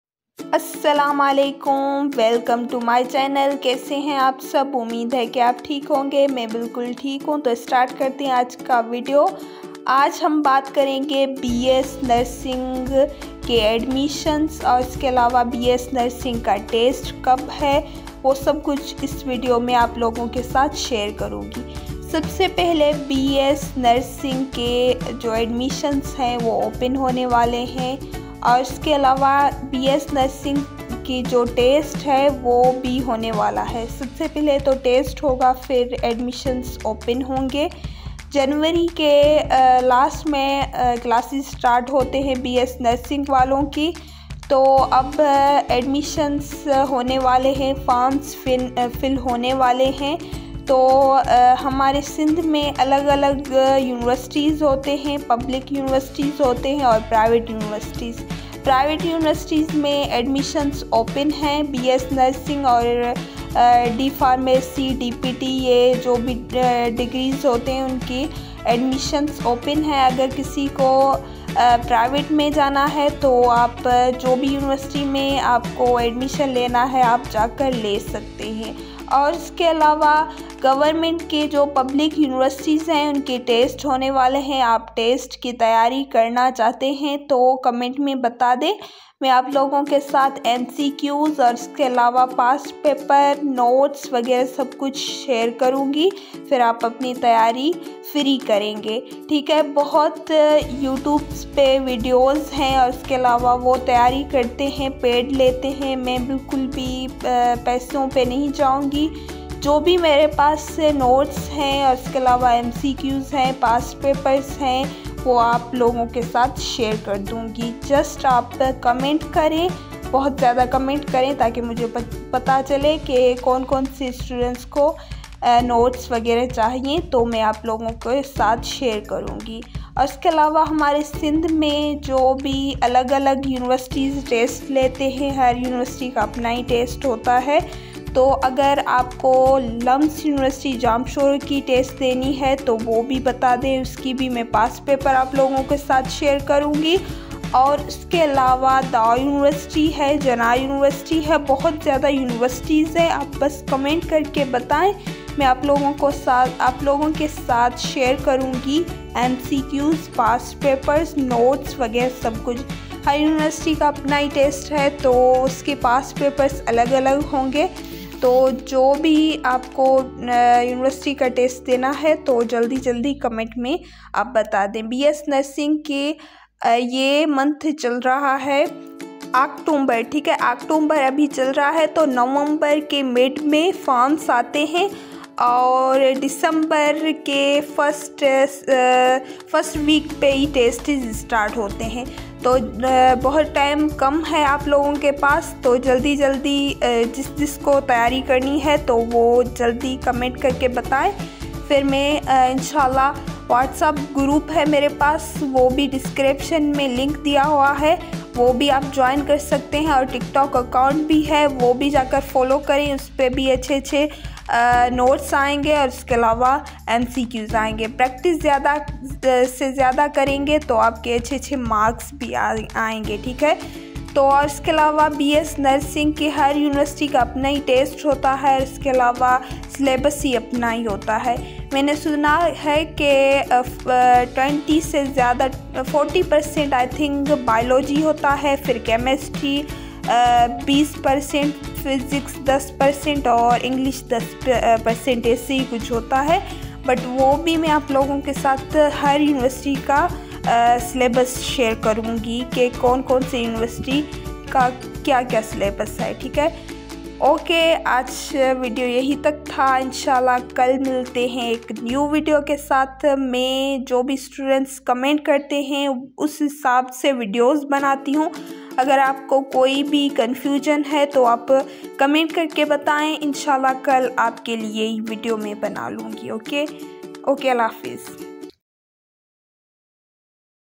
Bs nursing admissions open 2024 / how to test preparation start
Dear students, I hope you all are doing well.

Today I discuss about BS nursing Admission Test 2025